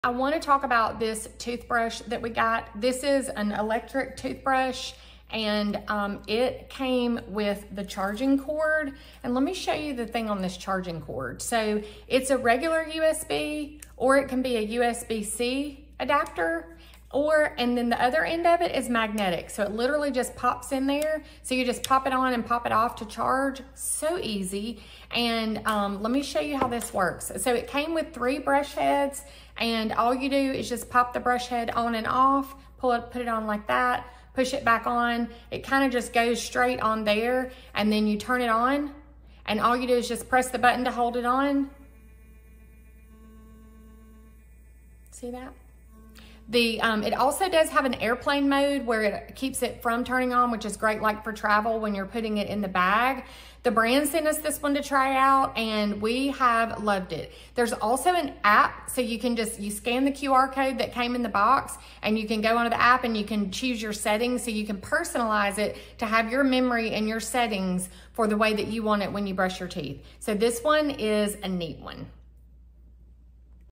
Review Laifen Wave Electric Toothbrush
Today, I am showcasing Laifen Wave Electric Toothbrush, Oscillation & Vibration Sonic Electric Toothbrush for Adults with 3 Brush Heads, IPX7 Magnetic Rechargeable Travel Powered Toothbrush (Silver Aluminum Alloy)